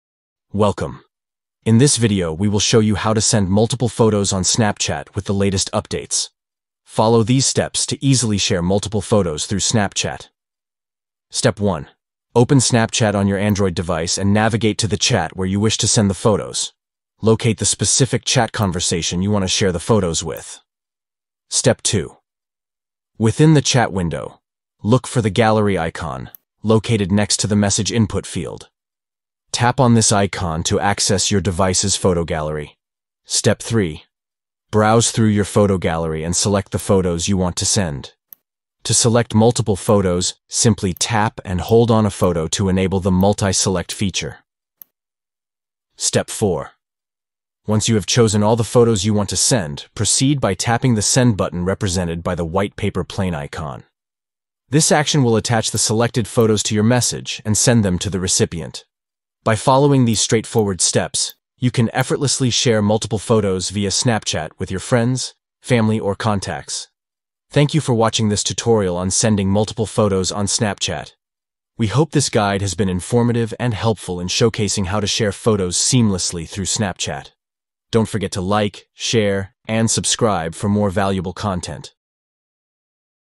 How to Send Multiple Photos On Snapchat (Updated)
Learn how to send multiple photos on Snapchat with these steps:

1. Open Snapchat on your android device and go to the chat where you want to send the photos.
2. Tap on the Gallery icon next to the message input field.
3. Browse and select the photos you want to send. You can tap and hold to select multiple photos.
4. Once you’ve selected all the photos, tap the send button (white paper plane icon).

Discover the updated method to effortlessly share multiple photos on Snapchat. Snapchat, MultiplePhotos,# AndroidTutorial.